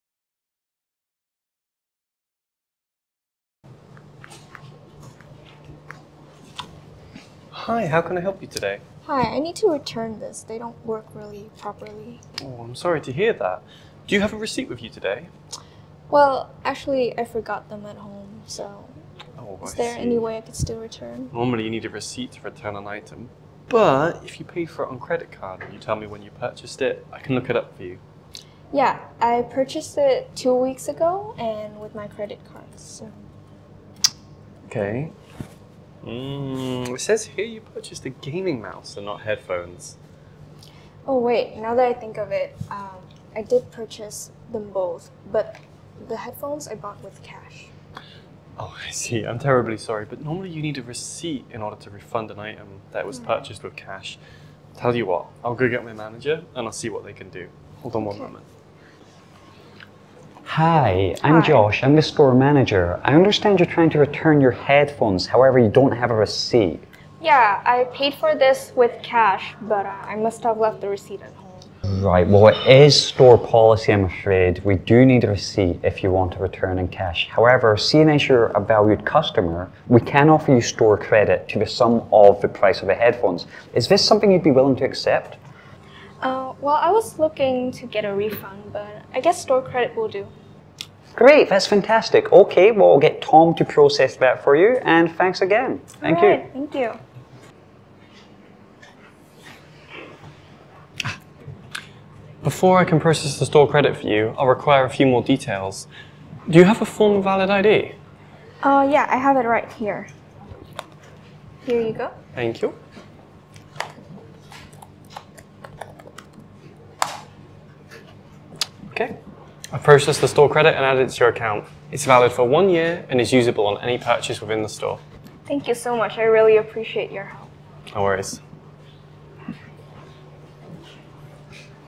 Lisa visits the shop to return the newly-purchased headphone.
Lisa attempts to return a faulty headphone but doesn’t have the receipt. Tom can’t find the purchase in the system because it was paid for with cash. The manager, Josh, offers store credit instead of a cash refund, which Lisa accepts. Tom processes the store credit, and Lisa thanks them for their help.

👉 If you want to dive deep into the grammar, vocabulary, and language functions of this video, explore it in our Course on the eJOY English App 2!